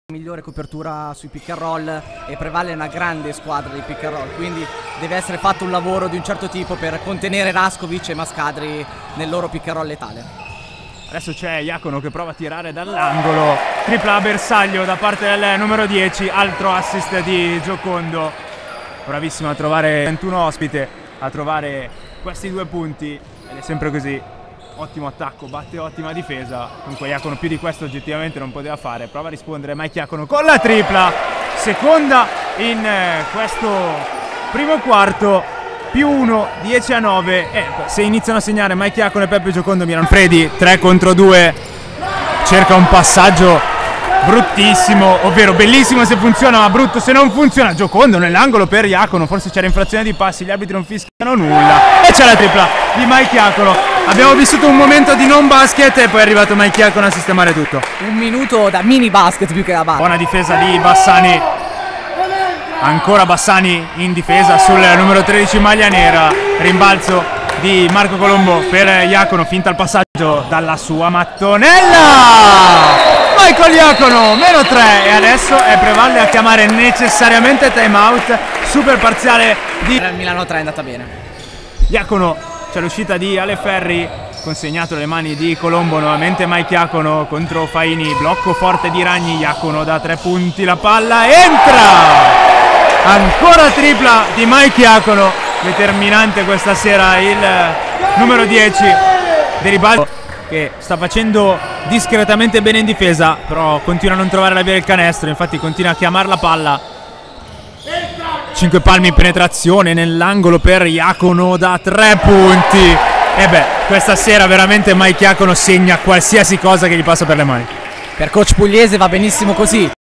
Milano3Basket.com: 6/6 da 3 di Michael Iacono
Le 6 bombe su altrettanti tentativi che il play dello Sporting Milano3 Michael Iacono ha realizzato nel match di Serie C Gold contro Prevalle